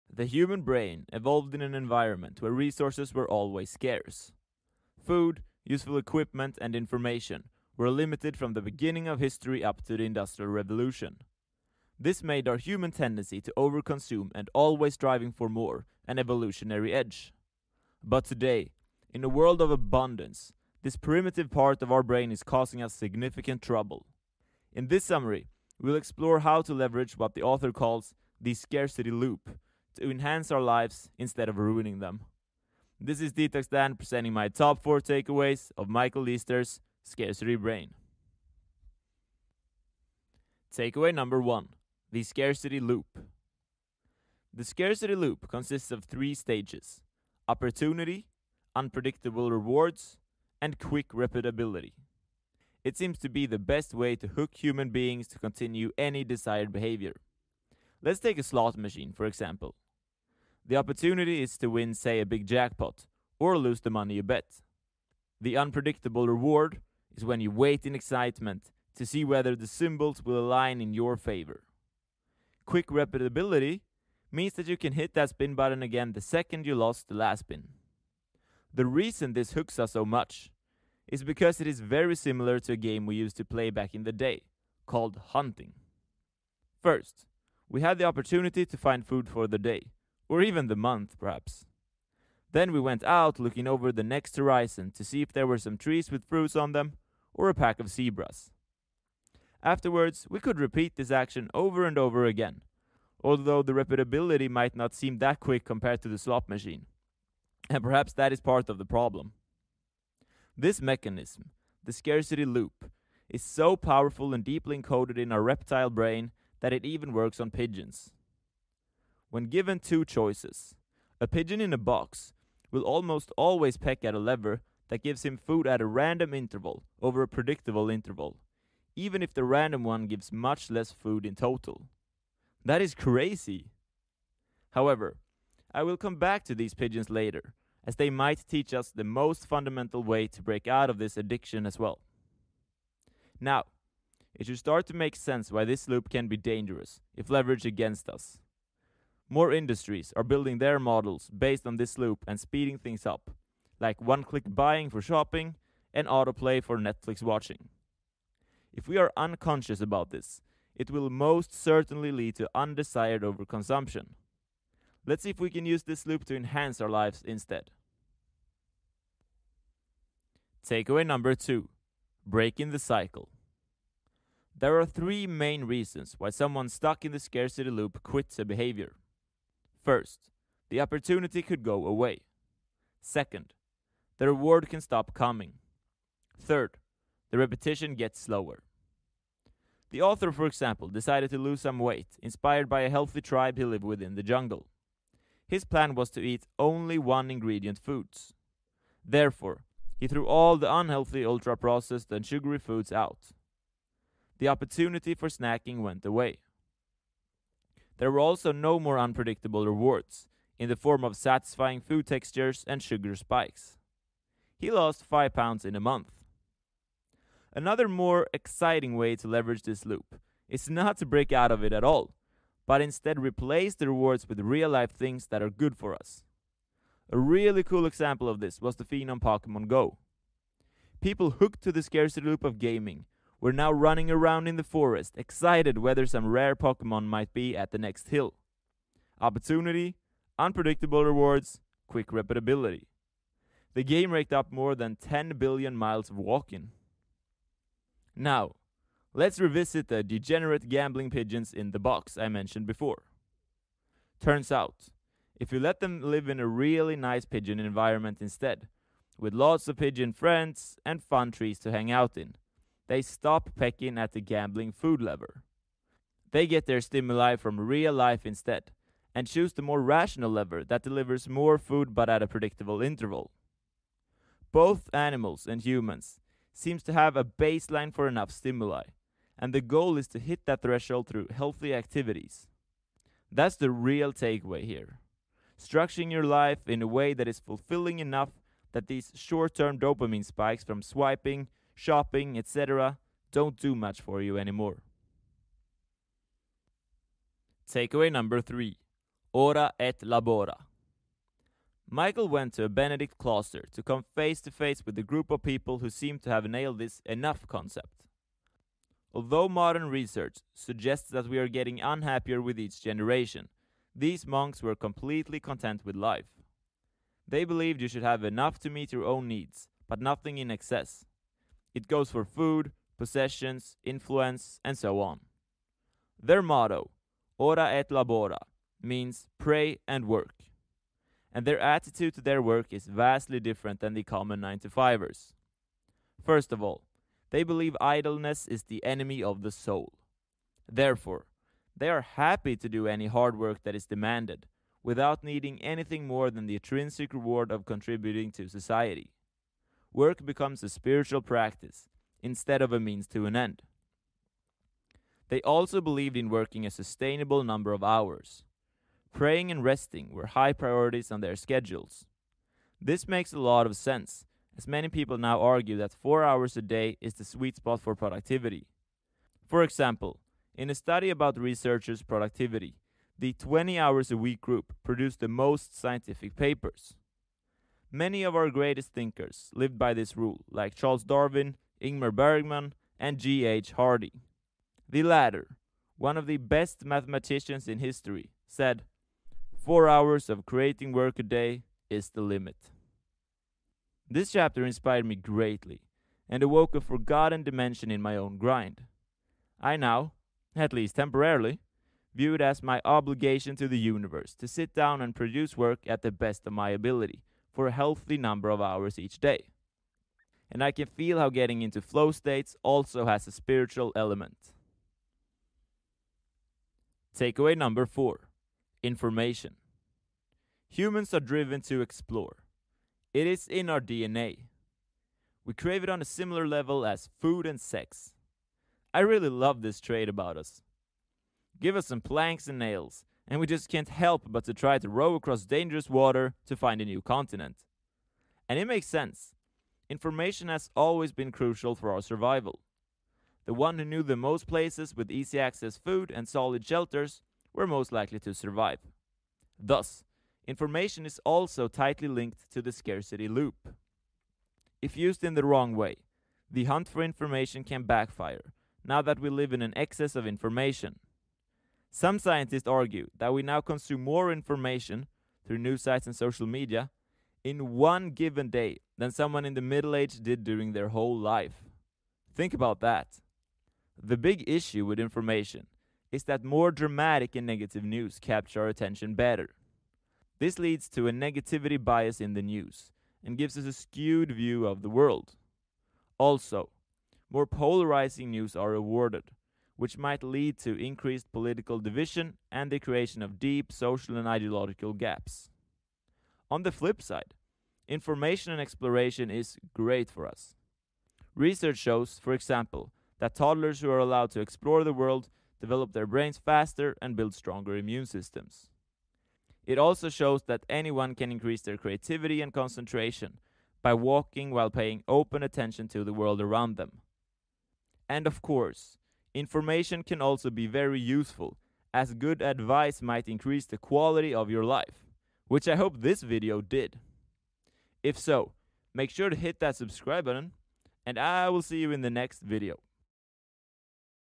SCARCITY BRAIN (BY MICHAEL EASTER)
This is a summary of Michael Ester's book Scarcity Brain. I have shared the top 4 most important takeaways from this book (in my opinion).